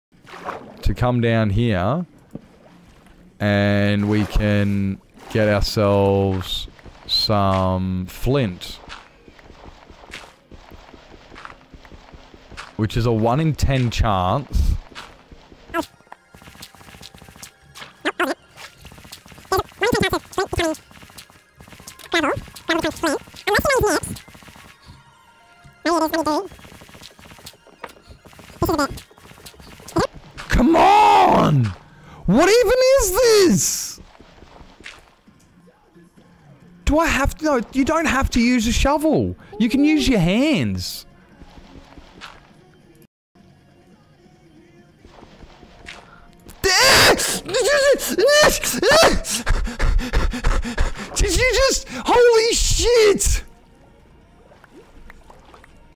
56 gravel for 1 flint!
trying my luck at speedrunning but RNG wanted in no way to help me.

Thank you to Epidemicsounds.com for the epic tunes